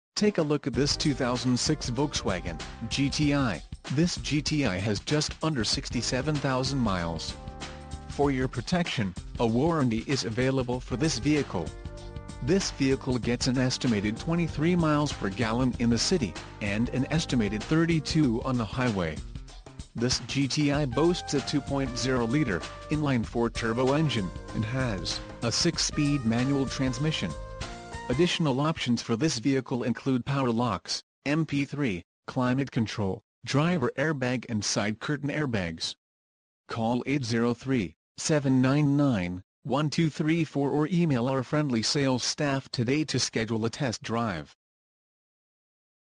2006 Volkswagen GTI Hatchback 2.0T Manual Hatchback - Columbia, SC
Jim Hudson Hyundai, Columbia, SC - 6-Speed Stick, 200 HP, Cruise, Side Airbags, 6-Disc CD w/MP3, Keyless, Much More